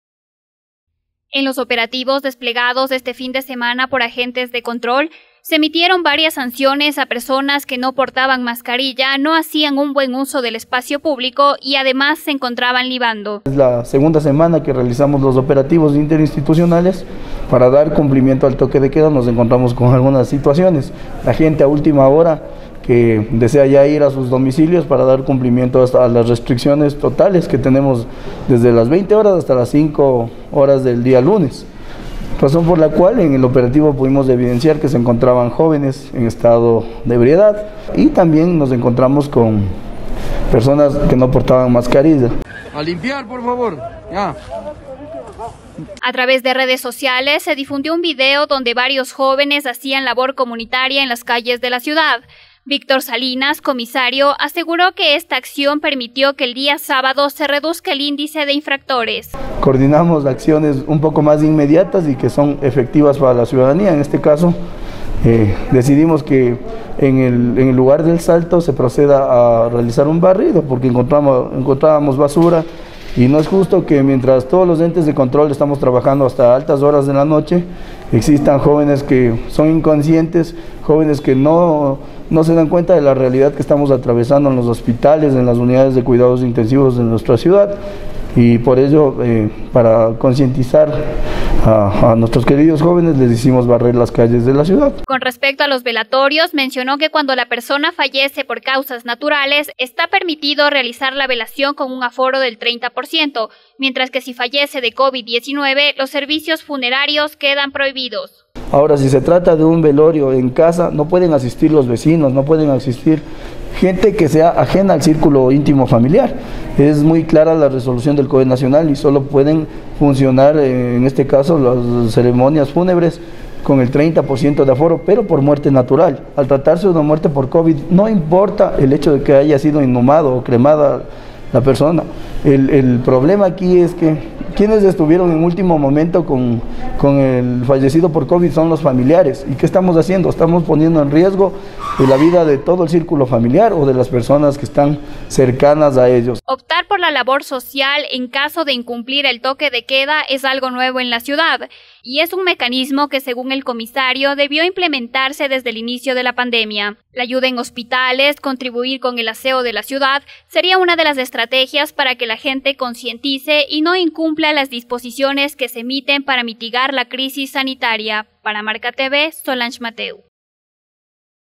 INFRACTORES AL TOQUE DE QUEDA REALIZARON LABOR SOCIAL, MIÉRCOLES 6 DE MAYO
La Comisaría Municipal de Latacunga a través de diferentes operativos desarrollados en la urbe registró la presencia de infractores al toque de queda, quienes fueron sancionados con actividades  de labor social como la limpieza de basura en las calles.